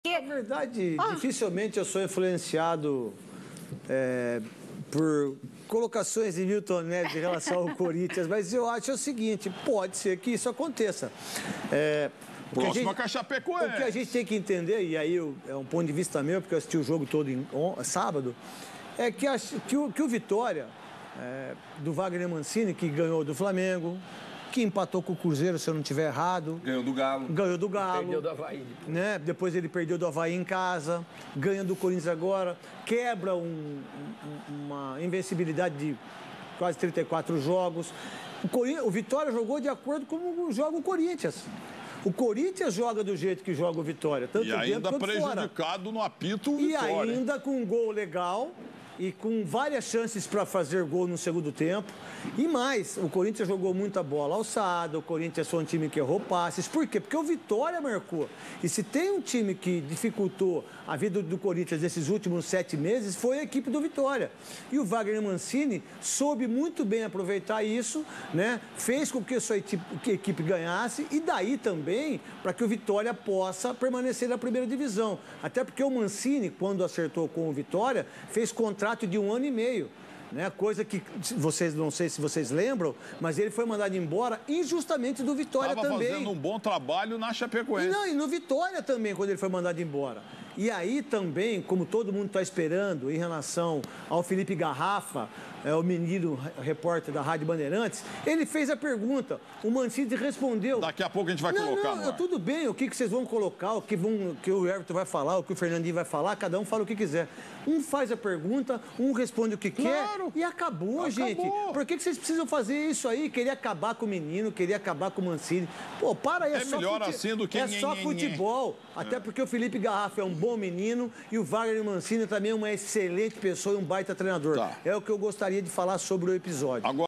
Neto sobre Vitória: dificultou a vida do Corinthians
O comentarista falou sobre o desempenho do time baiano no jogo contra o Timão.